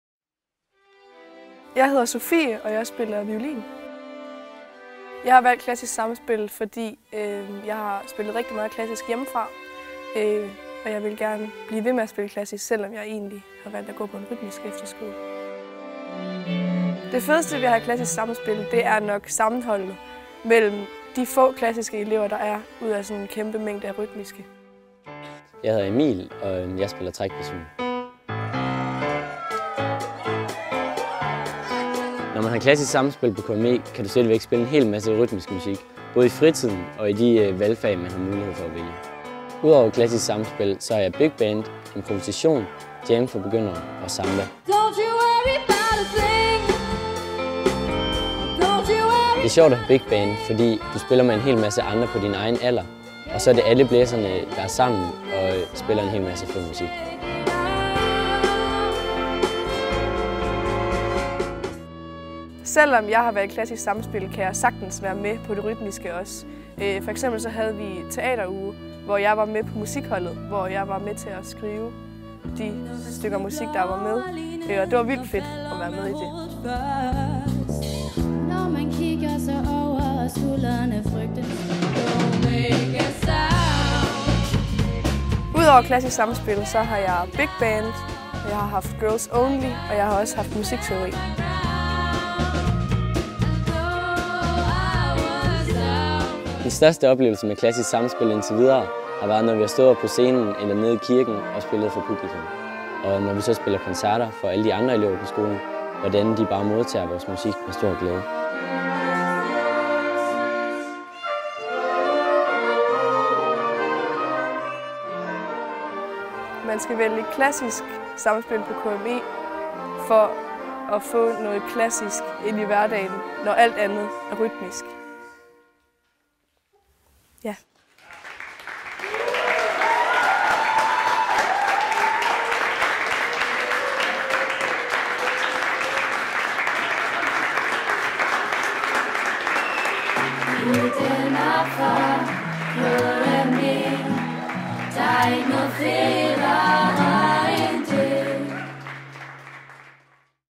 Klejtrup Musikefterskole - Hvorfor skal man vælge klassisk sammenspil?
Hvorfor skal man vælge klassisk sammenspil på Klejtrup Musikefterskole? Se og hør hvad de to klassiske elever Sofie og Emil mener om dette i denne video!
Vi tilbyder en bred vifte af fag du kan vælge imellem og du kan blande det rytmiske og det klassiske i en smuk pærevælling.
Se mere om skolen på eller find os på Facebook!